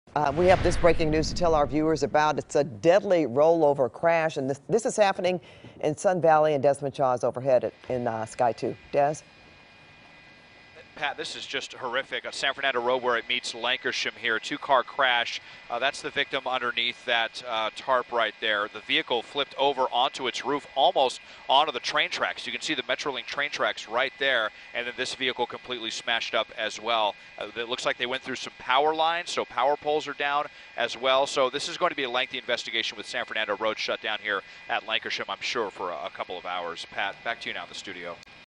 One dead after two-car crash in Sun Valley
One of the vehicles rolled over and landed near railroad tracks.